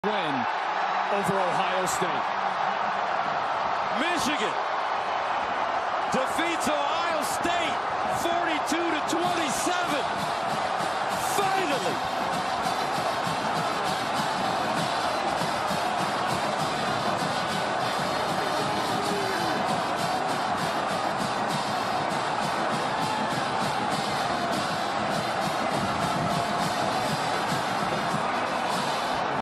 Michigan beats OSU for the first time in 10 years!
Jim Harbaugh is now 1-5 against OSU